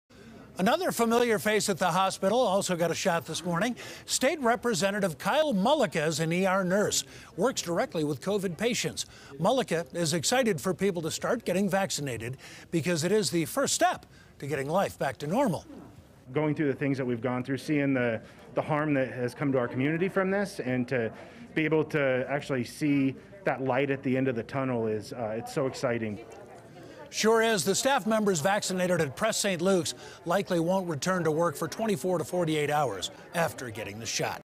State Lawmaker Who Is Also An ER Nurse Gets COVID Vaccination
State Rep. Kyle Mullica is an ER nurse and works directly with COVID-19 patients.